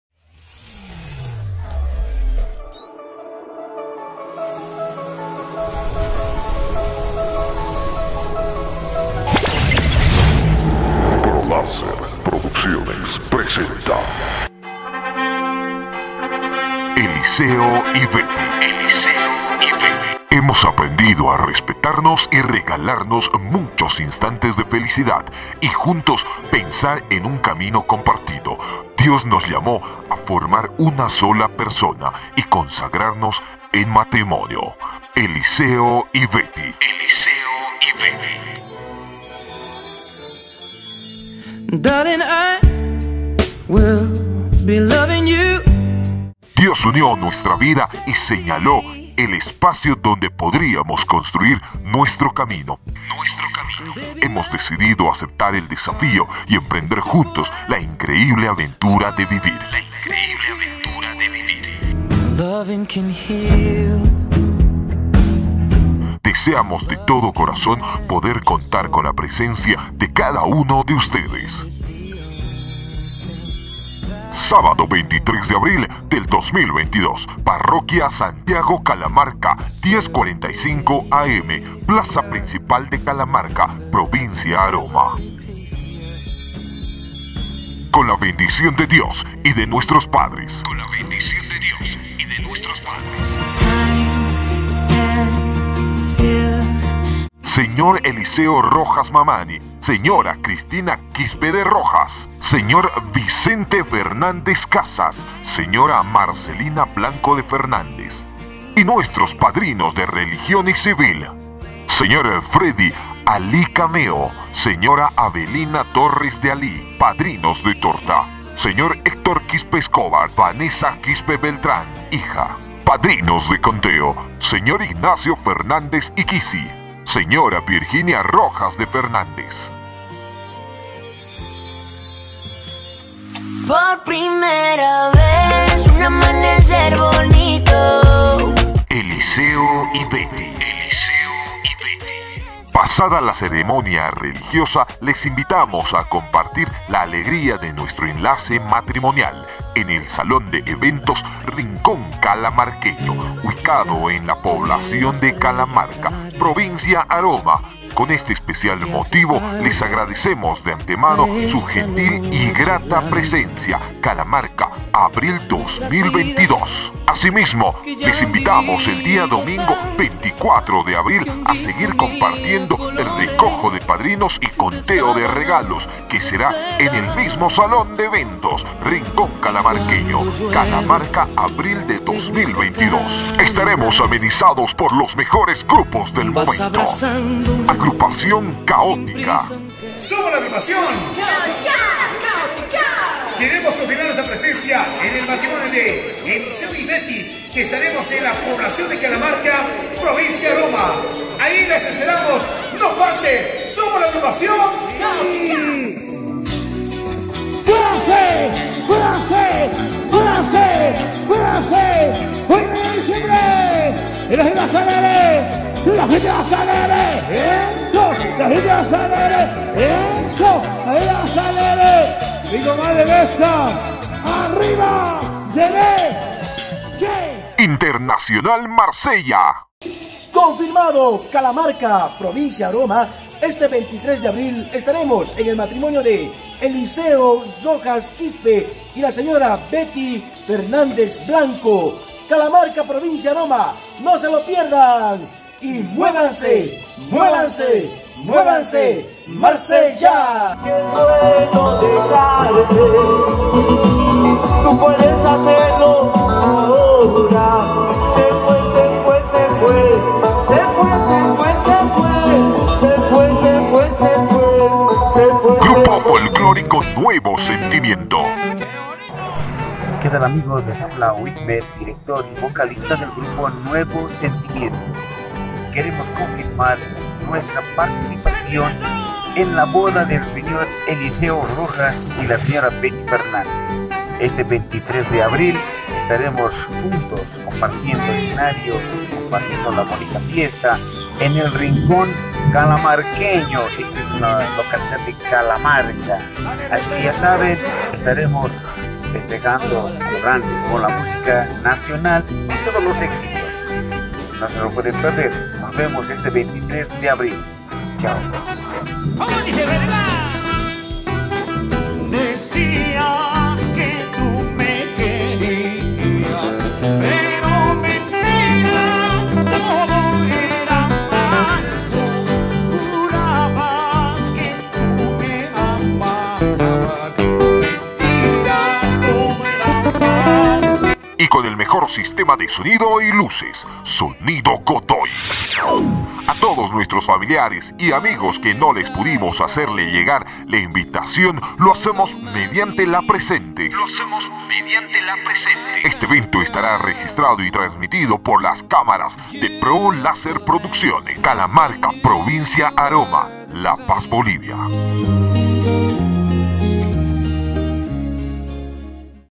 INVITACIÓN BODA - ELISEO Y BETTY 24/04/2022
𝐈𝐍𝐕𝐈𝐓𝐀𝐂𝐈Ó𝐍 𝐁𝐎𝐃𝐀 2022
💍🤵👰🤞𝐄𝐋𝐈𝐒𝐄𝐎 𝐘 𝐁𝐄𝐓𝐓𝐘💍🤵👰🤞
𝒂 𝒆𝒇𝒆𝒄𝒕𝒖𝒂𝒓𝒔𝒆 𝒆𝒍 𝒅í𝒂 𝑺á𝒃𝒂𝒅𝒐 𝟐𝟑 𝐀𝐁𝐑𝐈𝐋, 𝑯𝒓𝒔. 10:45 𝑺𝒂𝒍𝒐𝒏 𝒅𝒆 𝒆𝒗𝒆𝒏𝒕𝒐𝒔 "RICON CALAMARQUEÑO"
CALARMARCA  - LA PAZ - BOLIVIA